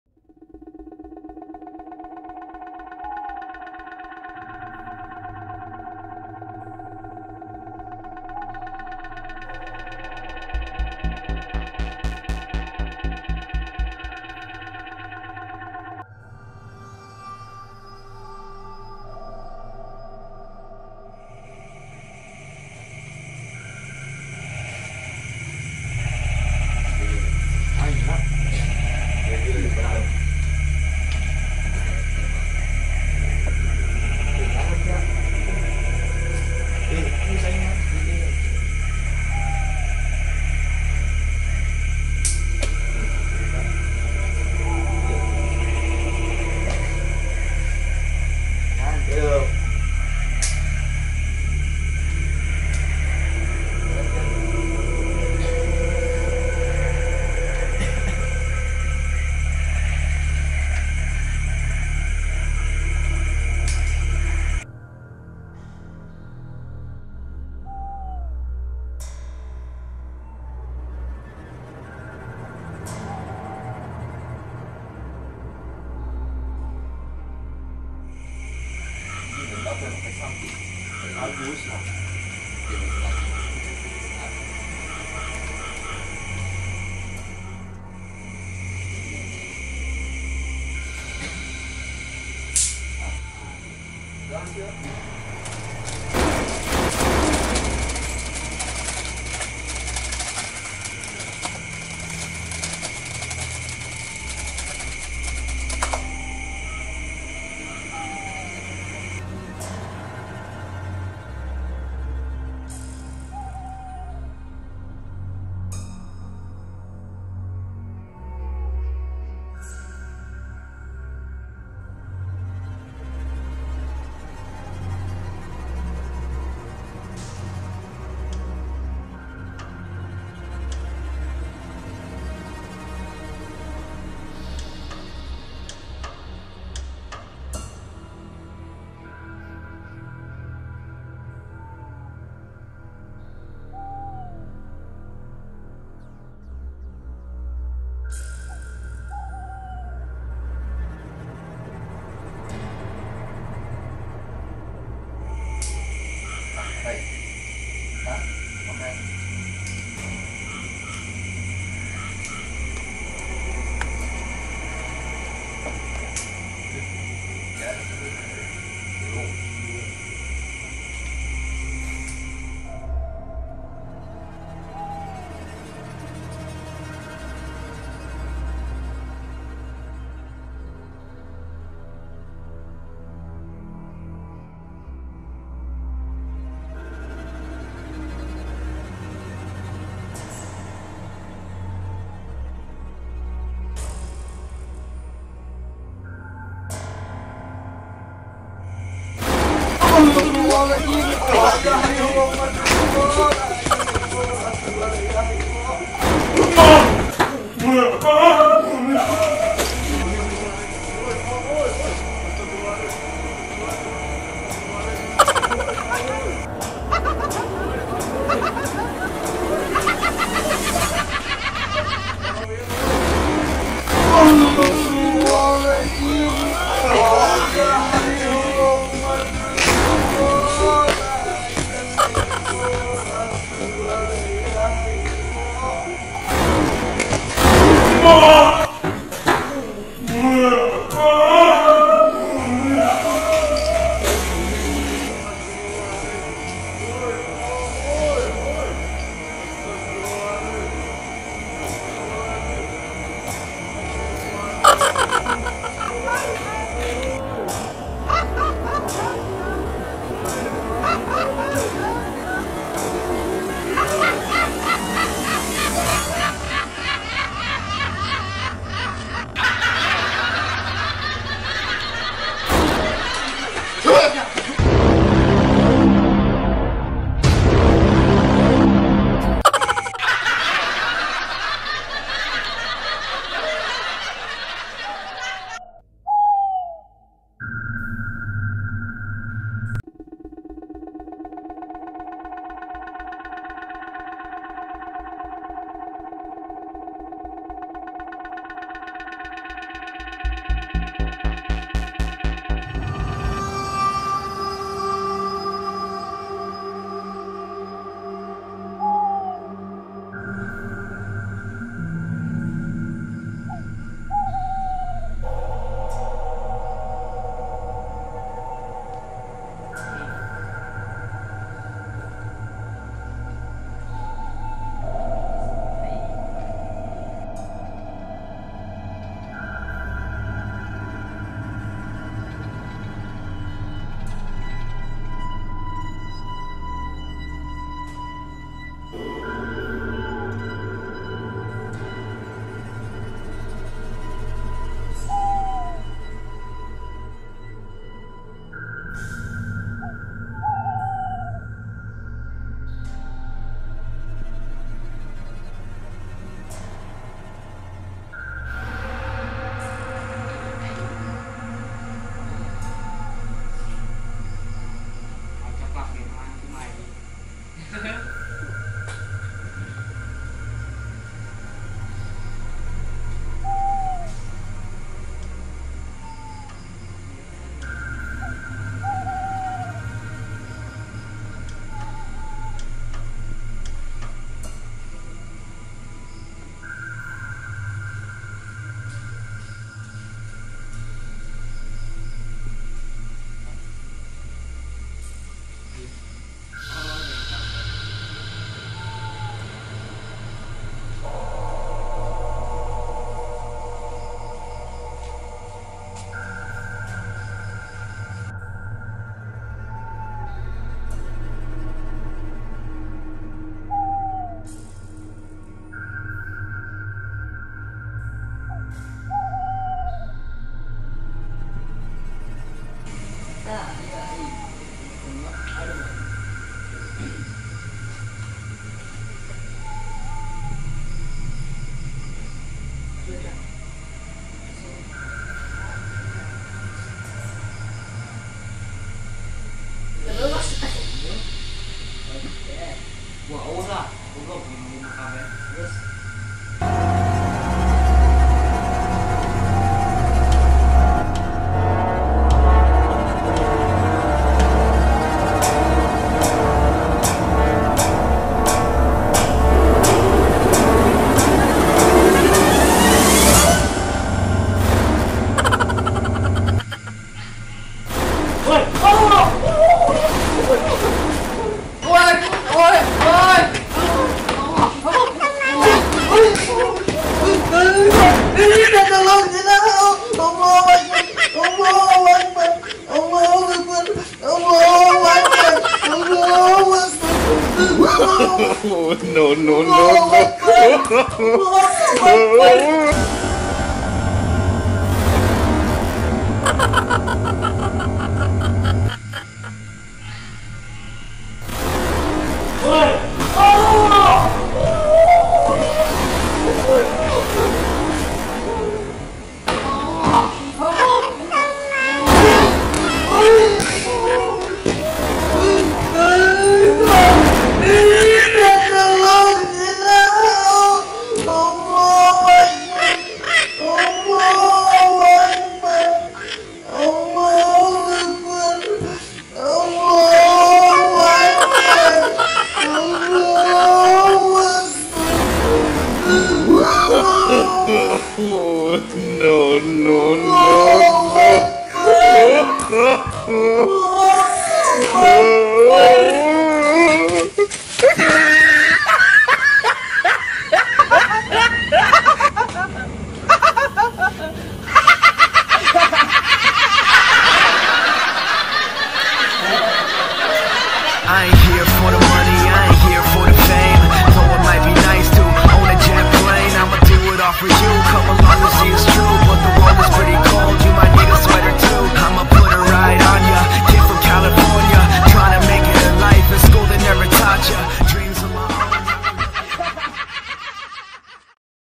301 ▶️ Asik Main Kartu di...🤣🙏 prankkuntilanaklucu funny Video Lucu
Hallo Kawan 🙏😇
Selamat Datang Di Channel ExCis ( Expose Cisoka ) Di video kali ini kami membuat video prank Kuntilanak yang suka gangguin Orang lagi Maksiat TerGila, Terjahil, Terngakak, Tergokil !!!
Saksikan keseruannya Semoga Terhibur Salam Ngakak 🤣🙏😇